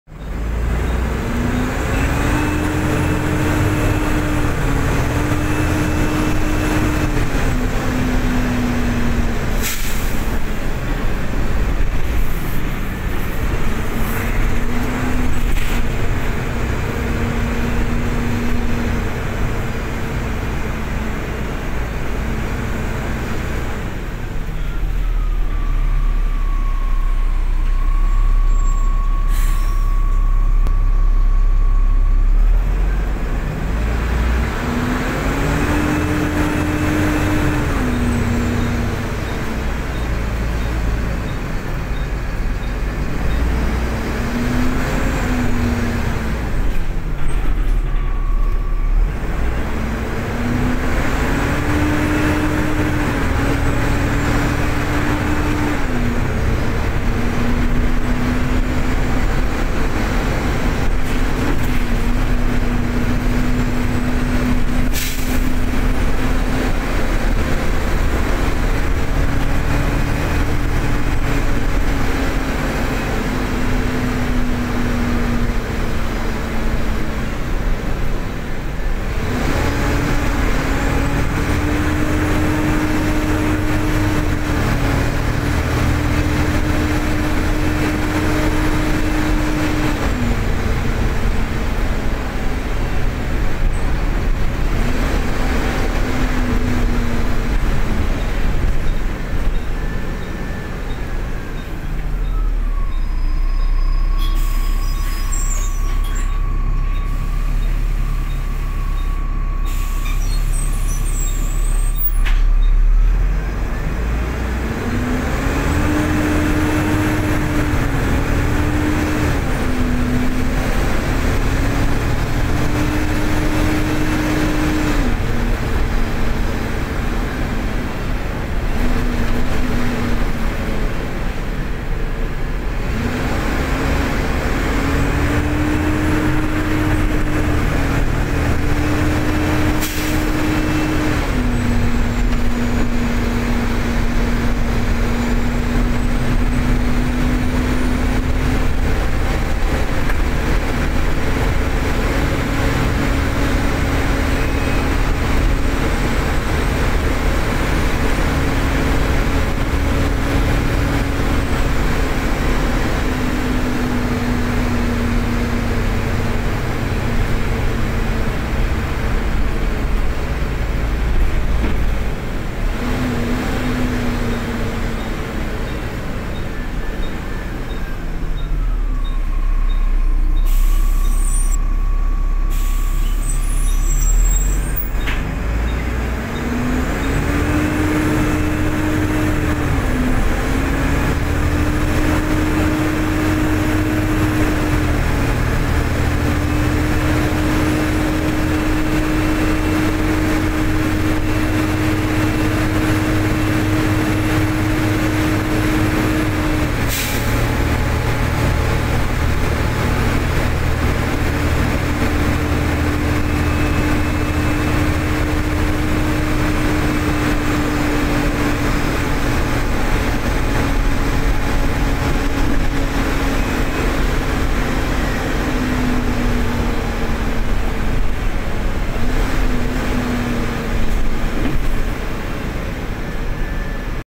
First Huddersfield Alexander ALX400 Volvo B7TL Y798 XNW (30943) on the 314 to Holme 10/03/2020
This is the oldest public service vehicle still operating in Huddersfield with First.